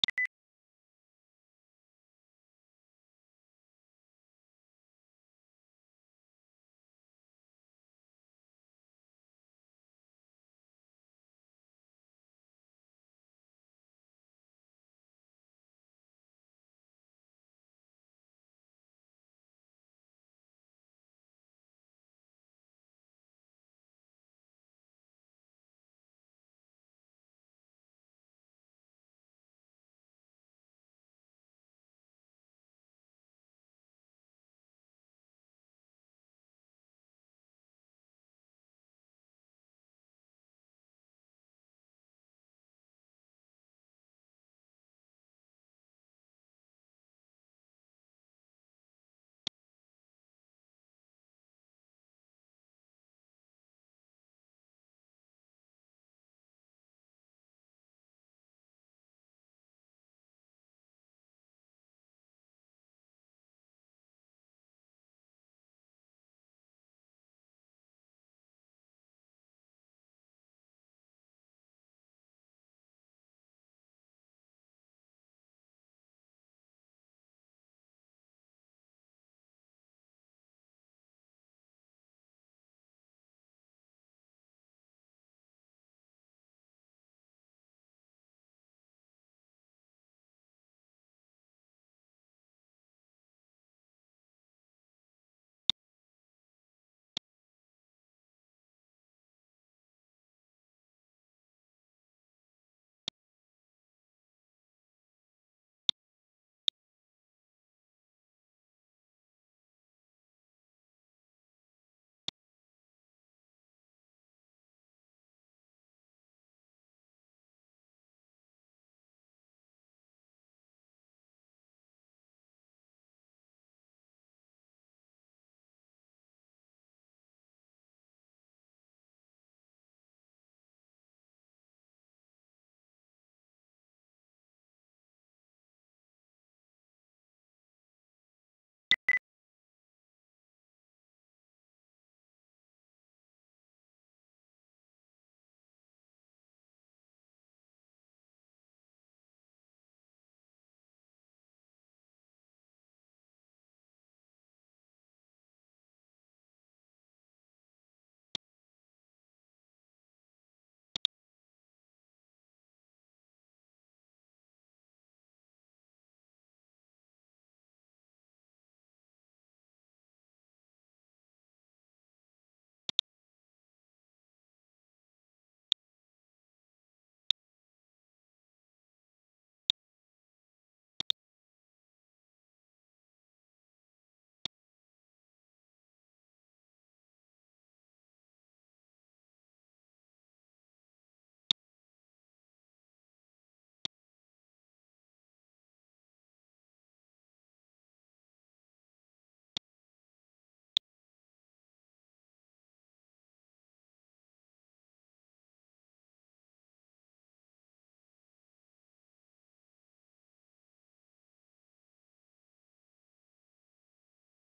BPSK demodulation, morse decoding, and subsystem monitoring
recorded at the NTU Aerospace laboratory